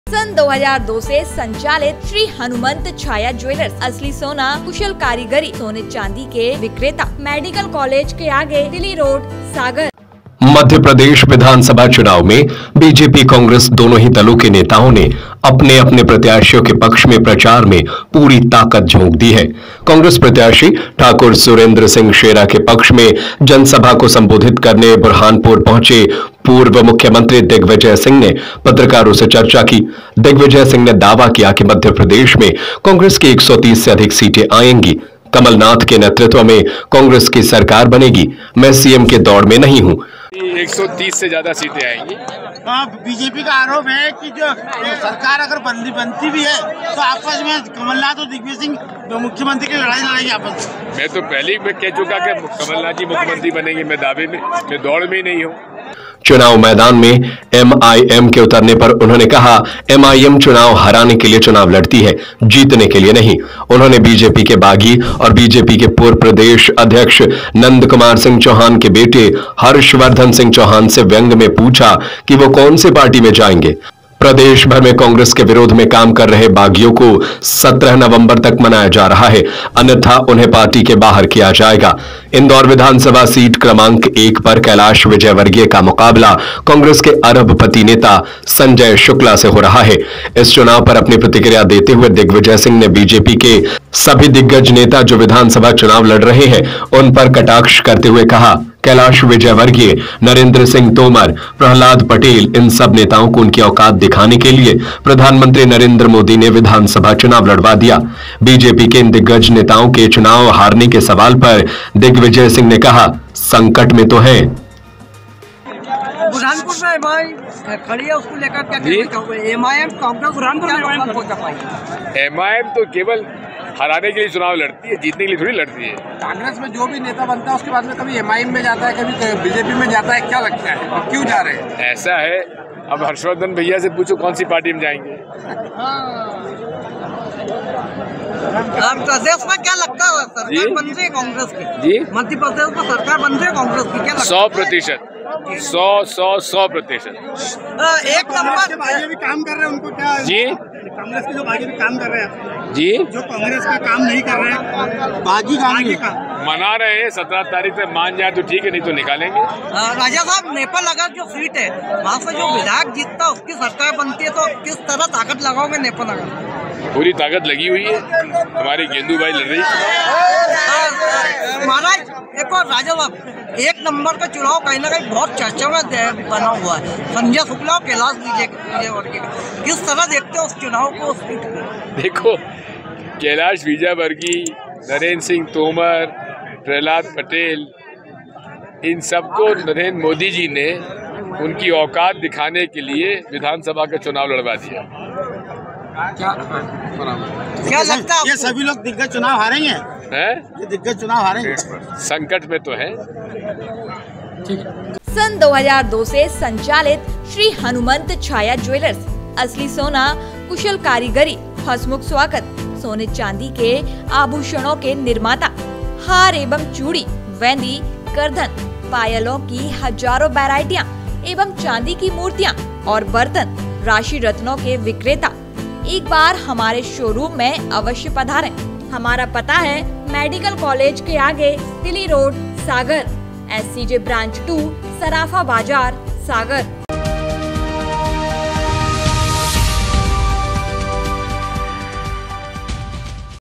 दिग्विजय सिंह का बड़ा दावा मप्र में कांग्रेस की 130 से अधिक सीटें आएंगी | sagar tv news
दिग्विजय सिंह का बड़ा दावा मप्र में कांग्रेस की 130 से अधिक सीटें आएंगी